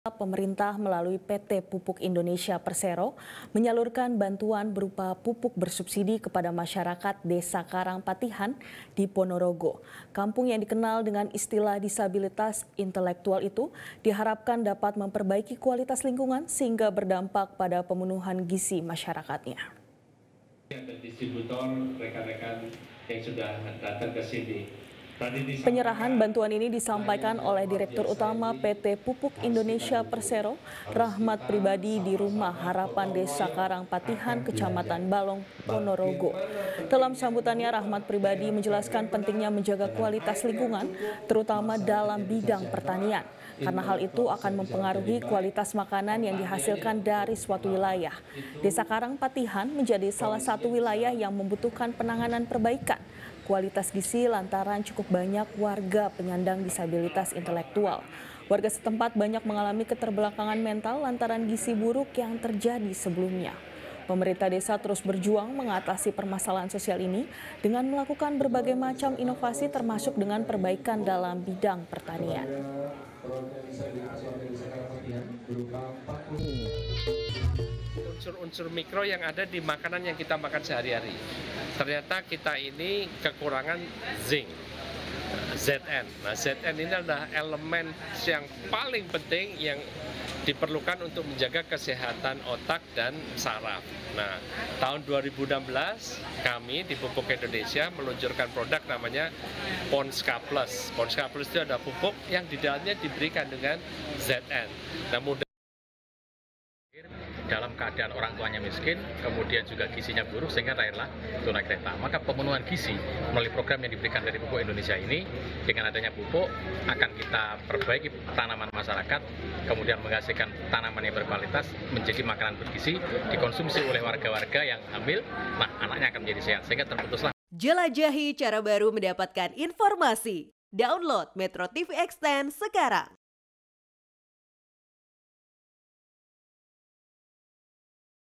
Pemerintah Berikan Bantuan Pupuk Gratis Untuk Desa Karang Patihan
METRO TV JATIM, pemerintah melalui PT pupuk Indonesia
Persero, menyalurkan bantuan berupa pupuk bersubsidi kepada masyarakat desa Karang patihan di Ponorogo kampung yang dikenal dengan istilah disabilitas intelektual itu diharapkan dapat memperbaiki kualitas lingkungan sehingga berdampak pada pembenuhan gizi masyarakat